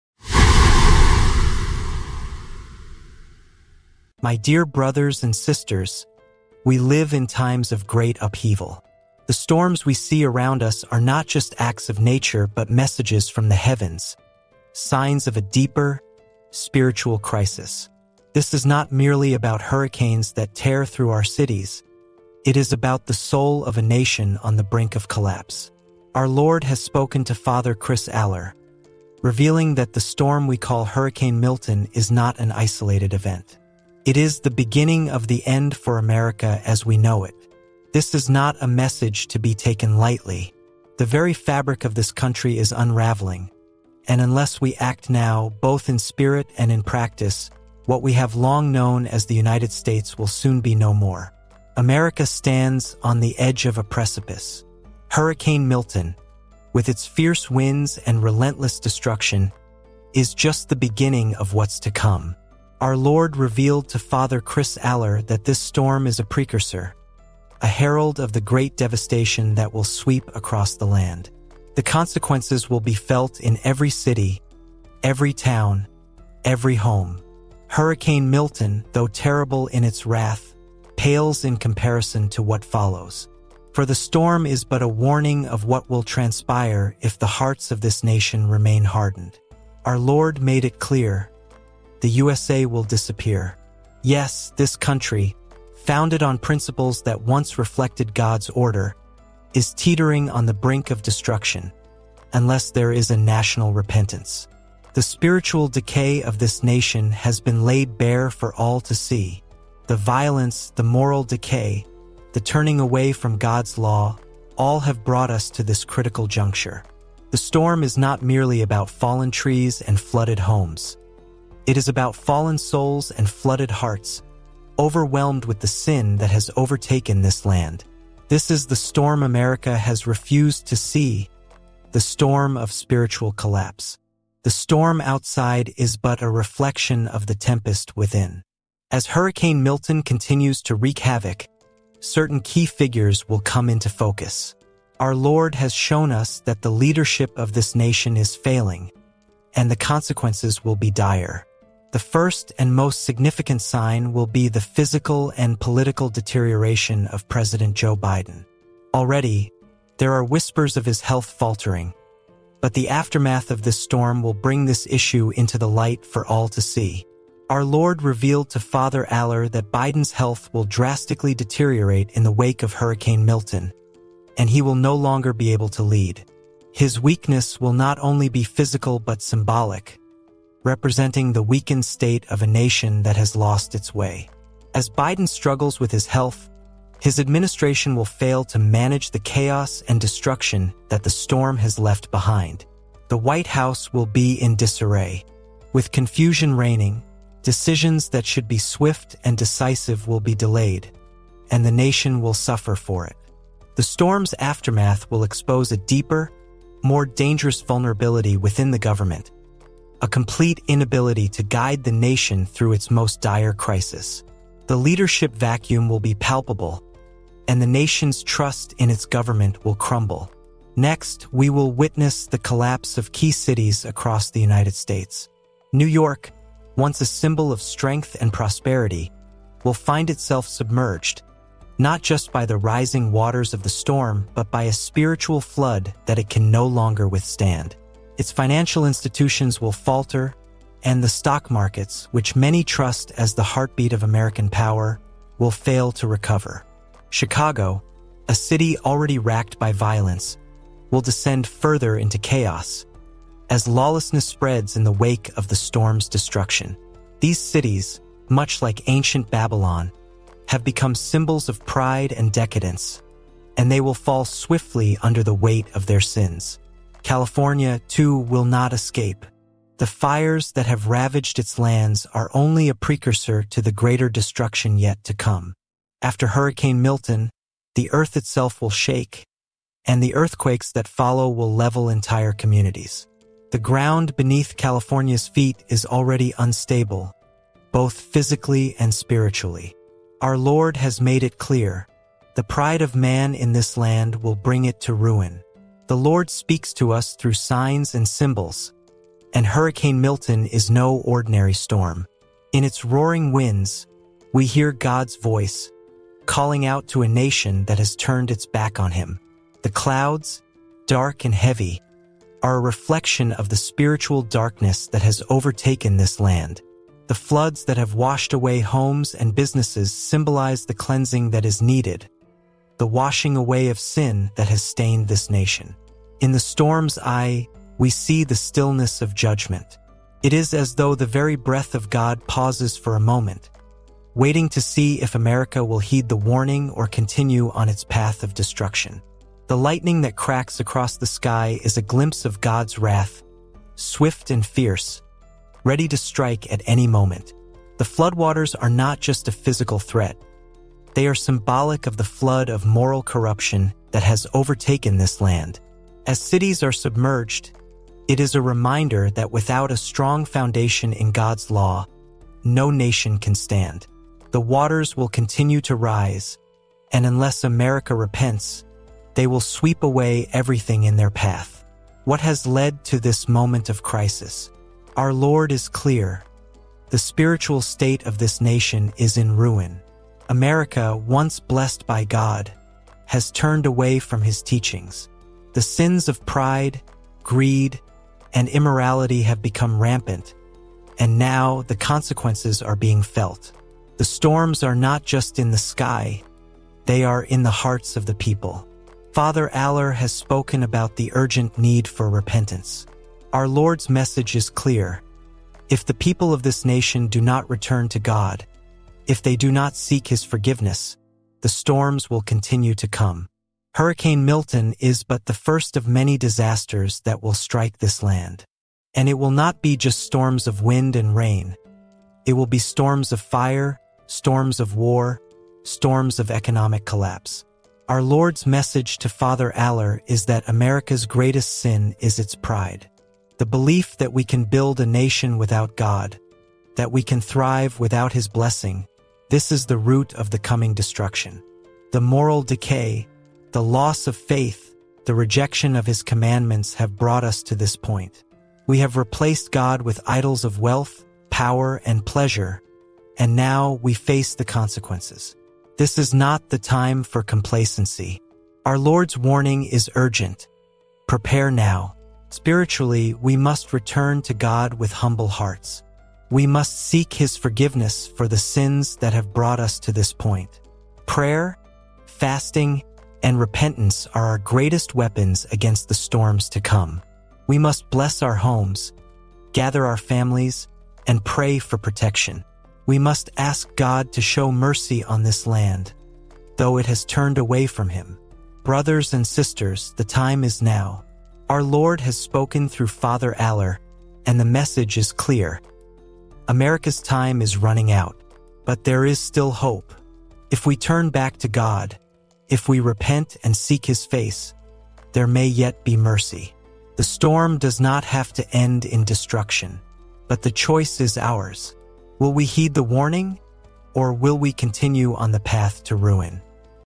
Fr. Chris Alar: In 11 Days, 3 Major Storms Strike USA, 24 States And This Man Will Disappear!
Father Chris Alar delivers a shocking prophecy about America’s future following the devastation of Hurricane Milton. Could the USA be on the verge of collapse, both physically and spiritually? With key cities and leaders facing unprecedented challenges, this urgent message reveals a deeper meaning behind the chaos. Will you heed the call to prepare before it’s too late? Discover the truth behind these warnings, watch now, and uncover what’s coming.